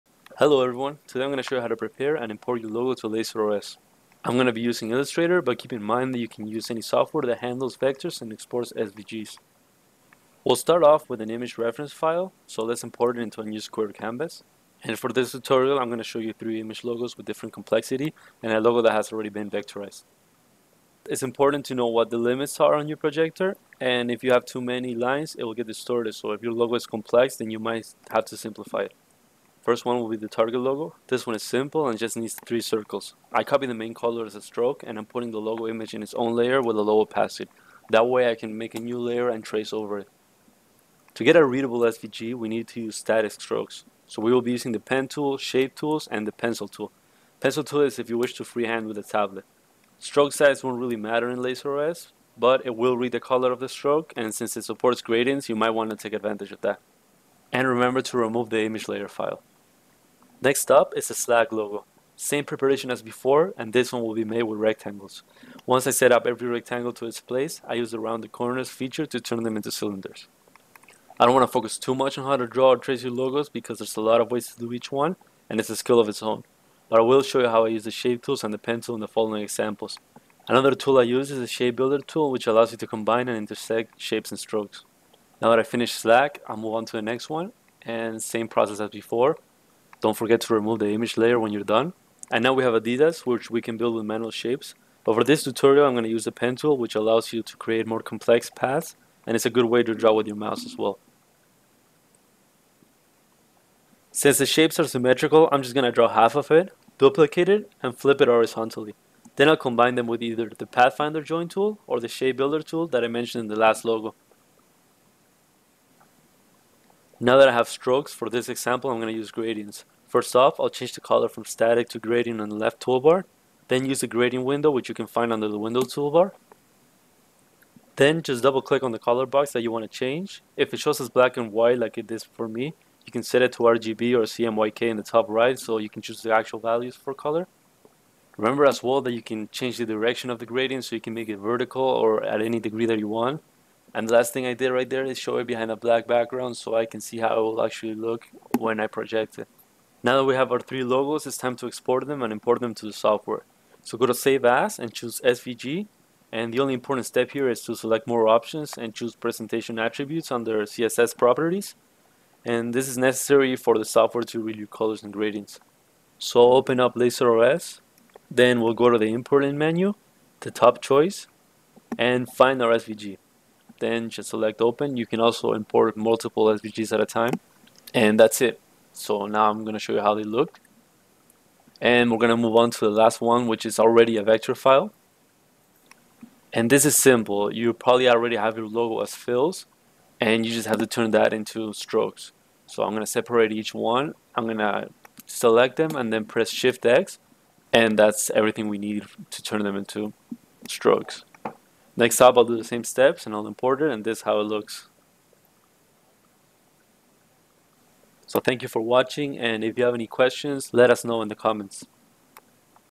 How To Create and Import a Logo for Laser Shows
How To Create A Logo In Under 5 Minutes | Logo Tutorial Wicked Lasers

The 3 pound, the 4-inch cube has bright red, green, and blue lasers and a computer-controlled mirror that can be used to display animations. Its beam can also be focused to engrave images or ignite things.